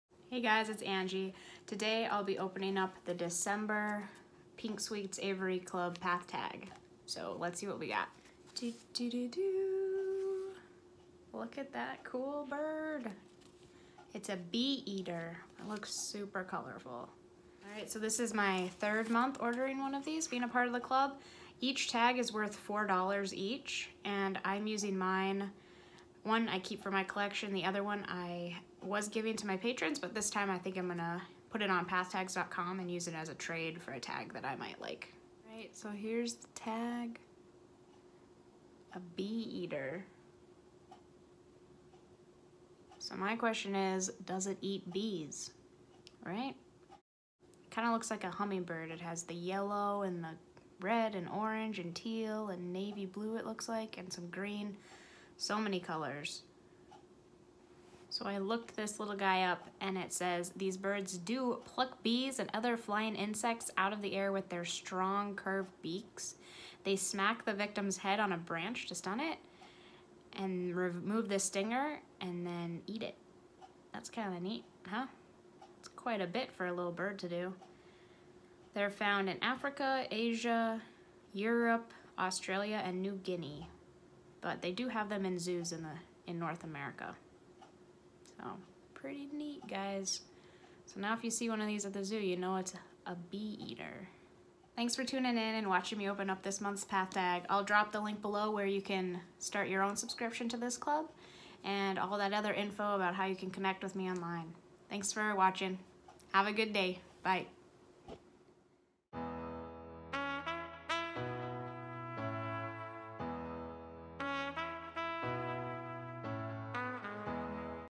Pink Sweets Aviary Pathtag Club | DECEMBER 2018
Hey everyone! Watch as I open the December 2018 PS Aviary Club's pathtag, plus I share a random fact about this month's bird - it's kinda intense! Have a great day.

✧Shop Pura Vida & Save 20%✧
Code: ANGIEWESTPHAL20